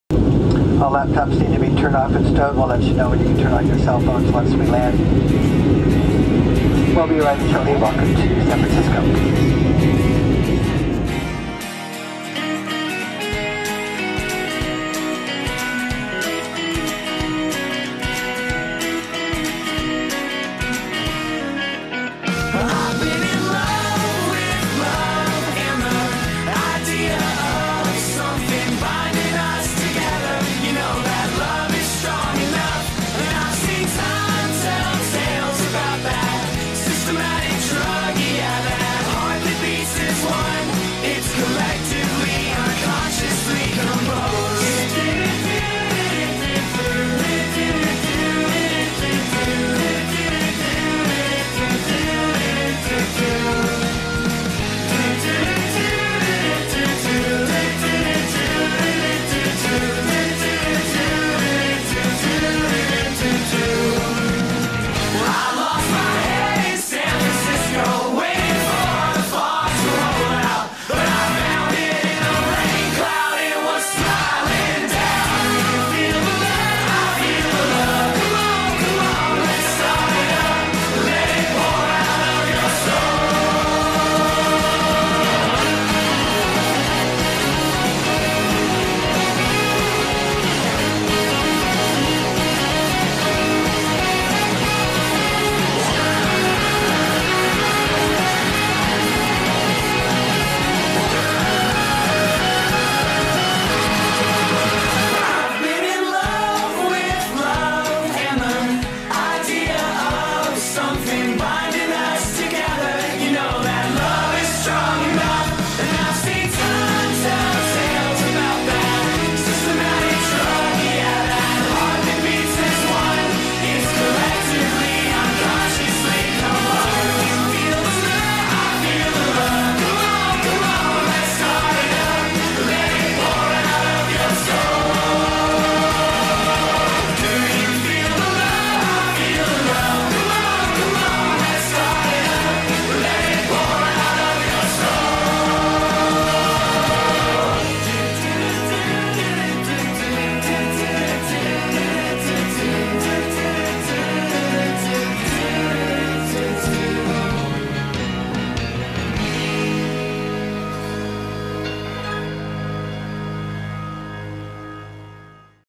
San Francisco 2019
First time in fog city was not so bad. A little sunshine, a little rain, but overall just right for walking around the city and seeing the sights. The food was good and the celebratory drinks were better! Happy birthday Olivia, see you again soon!